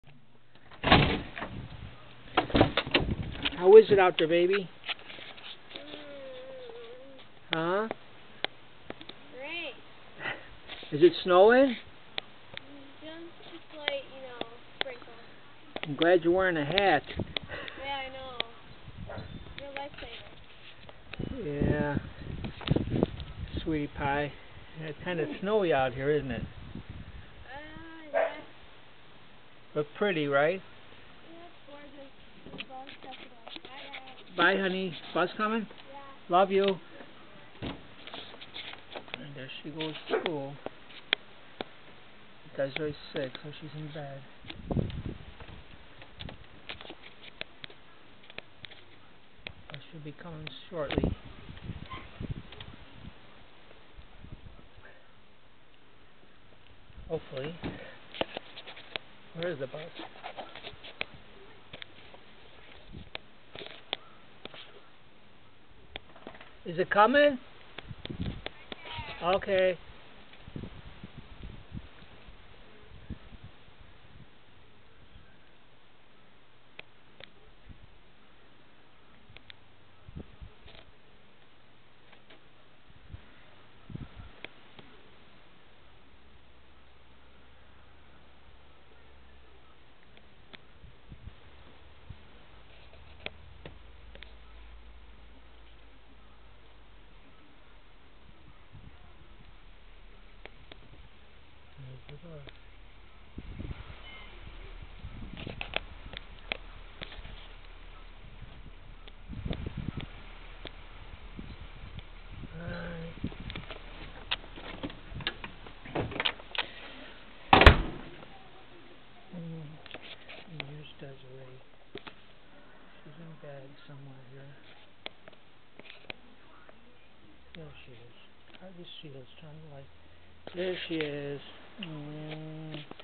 Dana and the snowstorm
school wasn't cancelled even though the storm of snow began